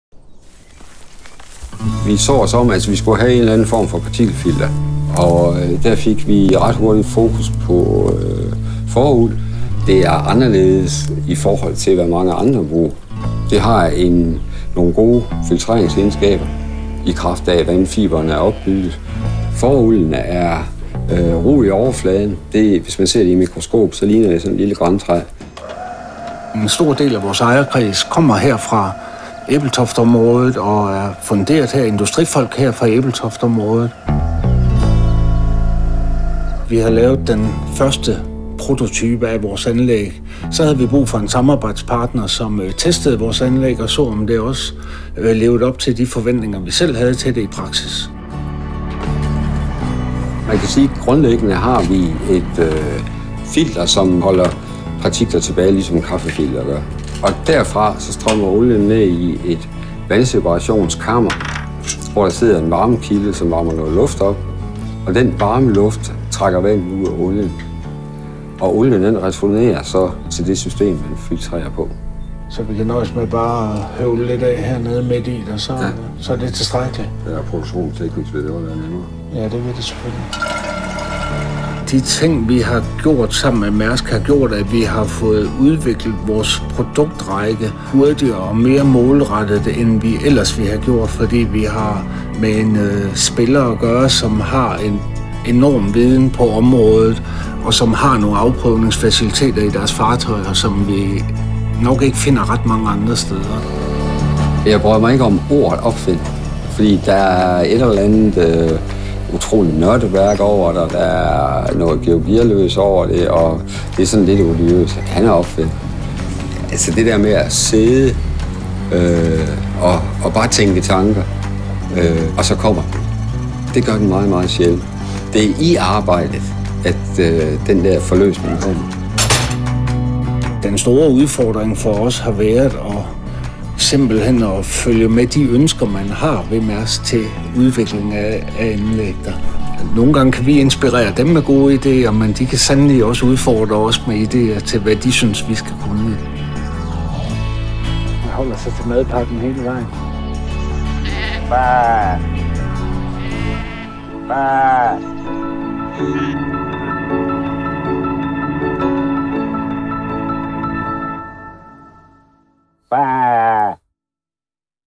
Maersk and the Danes – Sheep’s wool on the Triple-E
Many useful items can be made from sheep’s wool. An oil filter is not the first that springs to mind, but GreenOil, a small engineering company based in Ebeltoft, found it worked well because it has a rougher surface than the synthetic fibre commonly used.